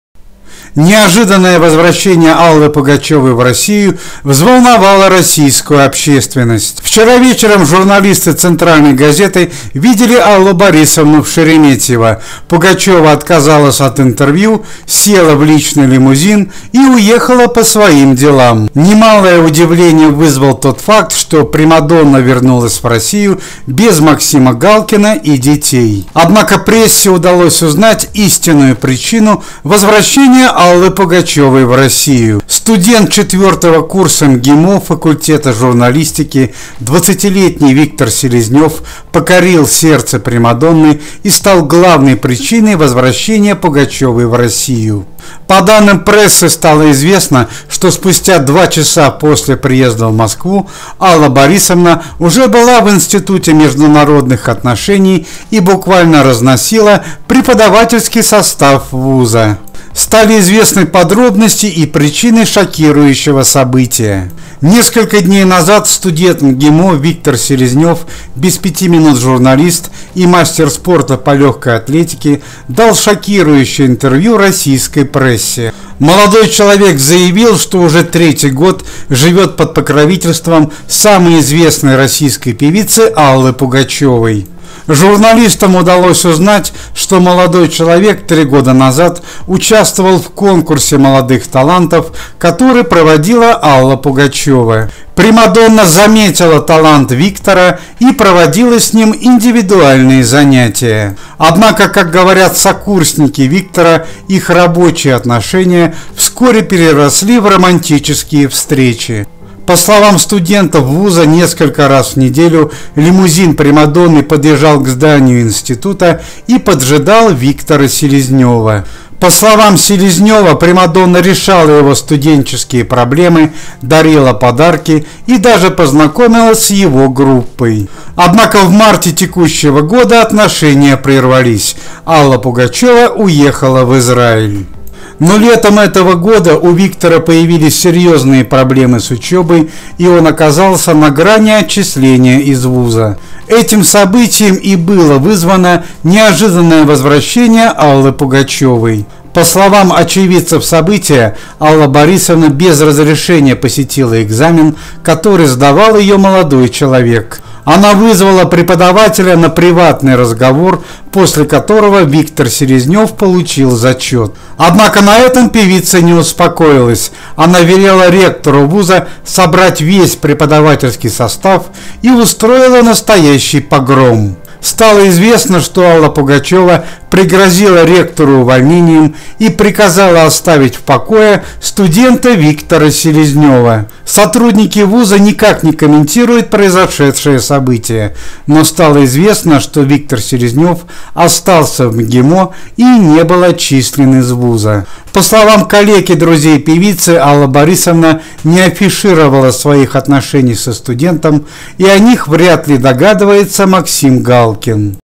10 минут назад / Пугачёва шокировала своим поступком , / "погром" в институте.
о известной российской и советской  певице Алле Пугачевой.
10 минут назад / Пугачёва шокировала своим поступком , погром в институте.
Вчера вечером Алла Пугачёва из Юрмалы вернулась В Россию, без мужа Максима Галкина
и детей.
Причина возвращения- новая любовь Аллы Пугачёвой- студент Мгимо 4 курса- Виктор Селезнёв.
В видео рассказываем подробности этого события.